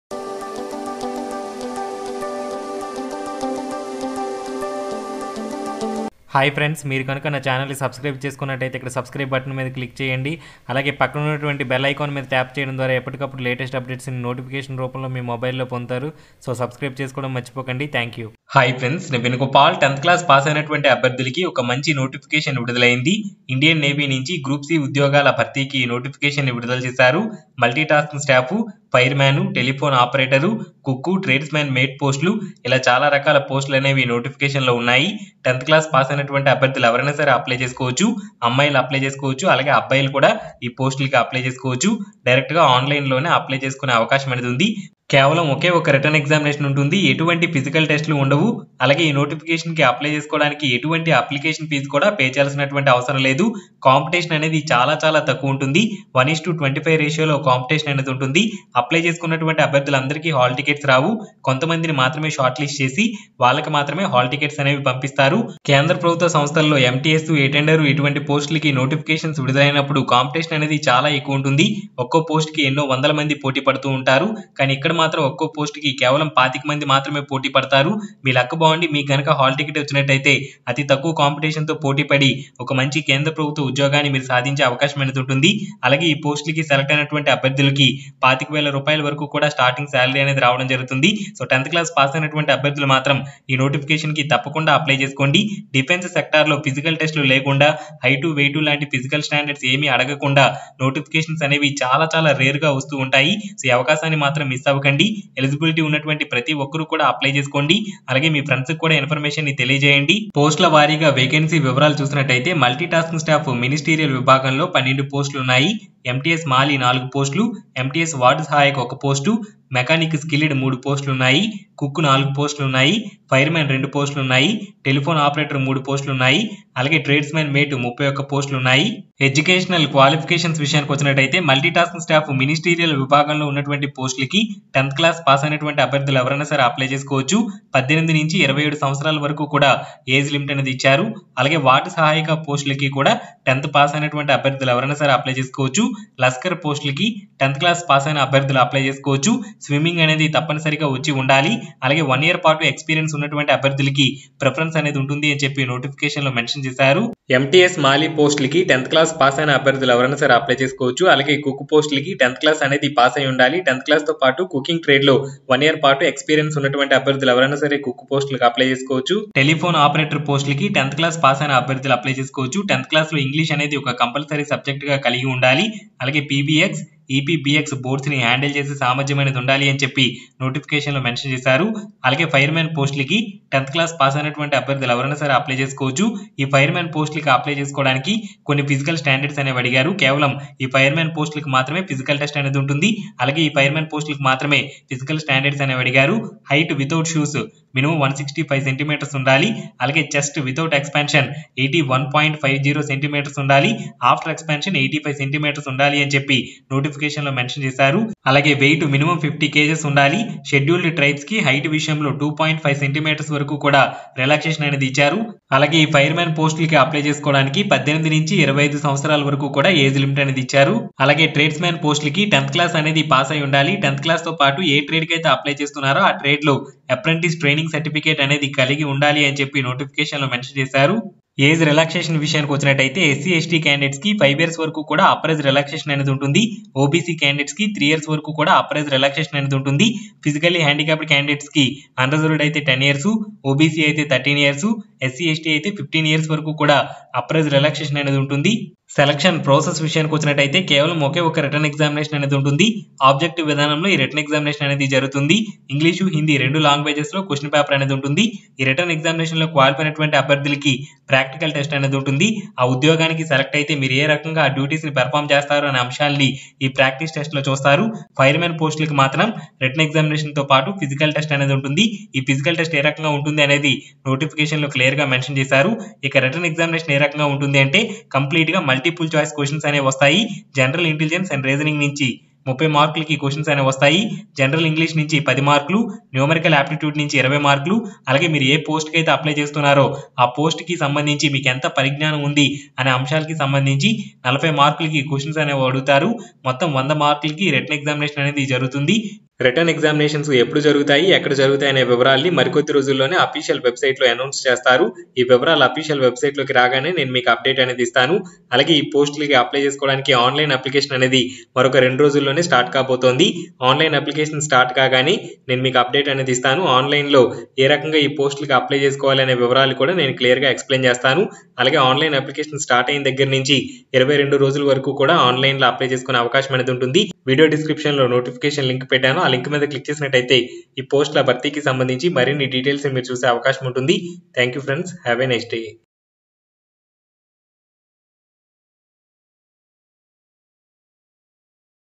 Government Jobs | Apply Online For Group C MTS Recruitment Notification 2018 | Job search
Recruitment Notification released for group c vacancies(MTS,Cook,Fireman and other). Watch this video for More details.